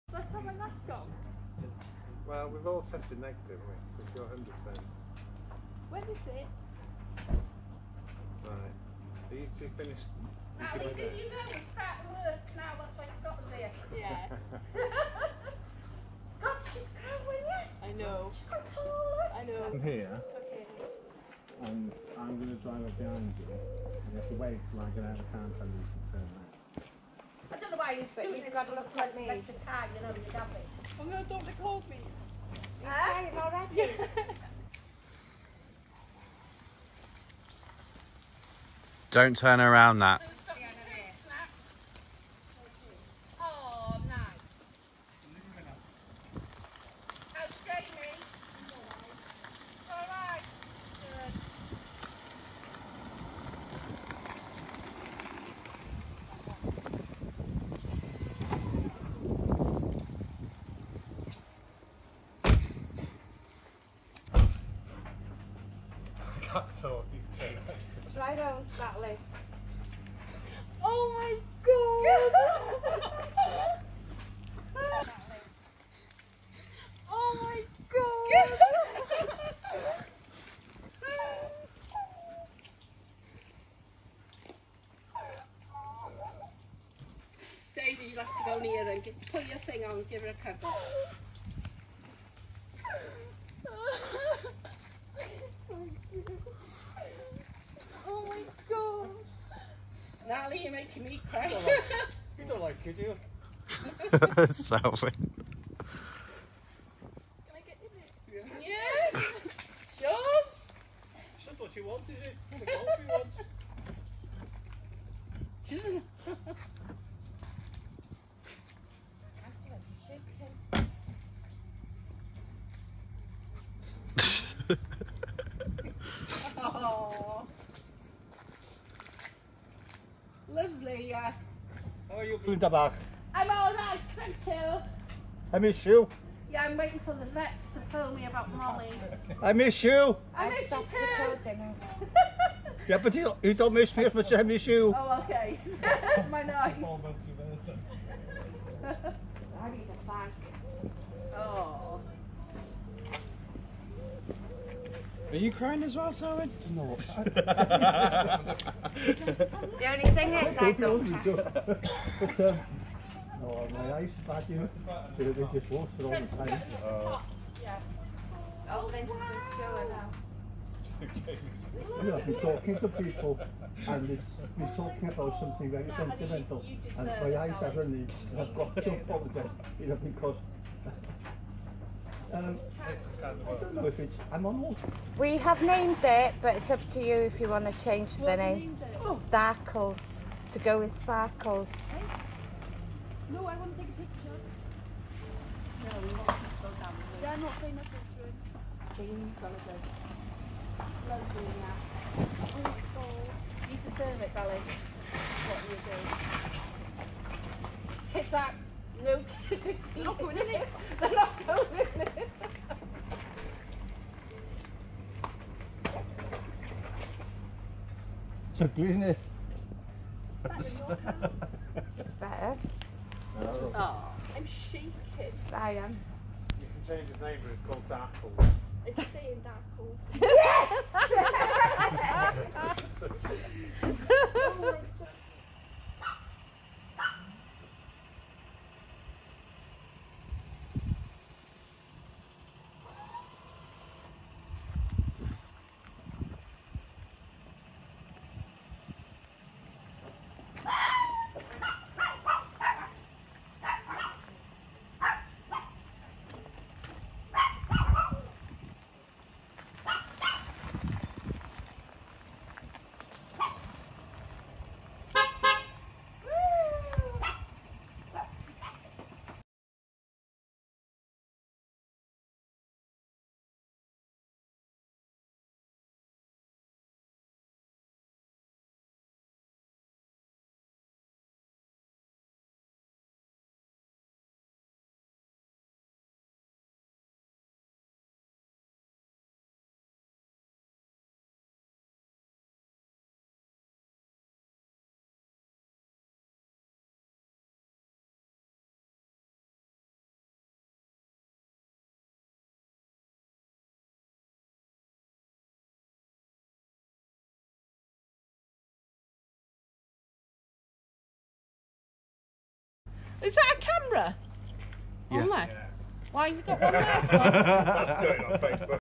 Surprise Tesla Model 3 for my niece
My Niece Natalie came over to collect her new car. She thought she was getting a 1 year old VW eGolf, she had no idea she's was actually getting a brand new Tesla Model 3 SR+.